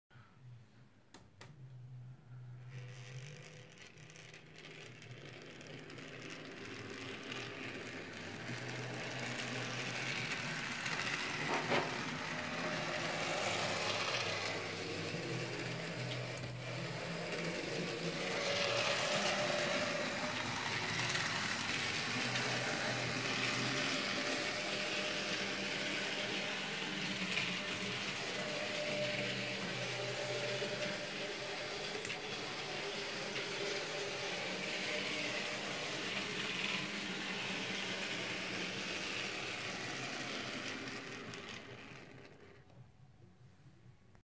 GFP-634-79327
Dapol N Gauge A4 been tested in DC after repair